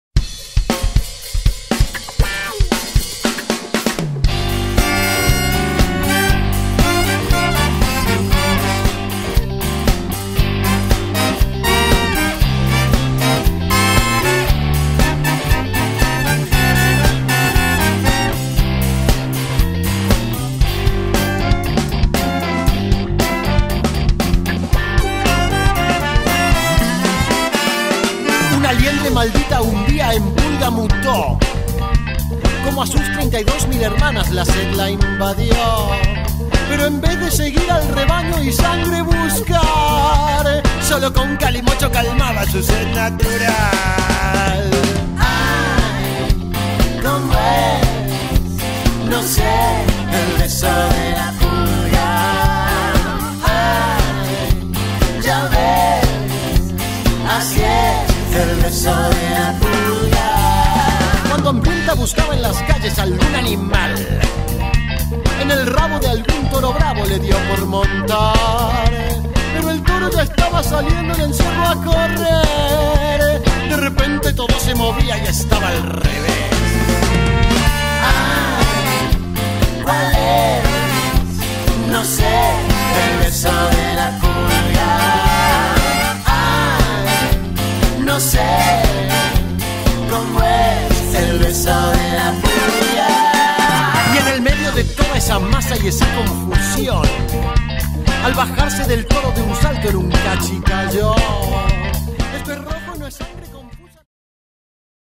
VAQUILLAS dia 7 de Julio 2008
Los mejores momentos de las vaquillas de Sanfermin 2008.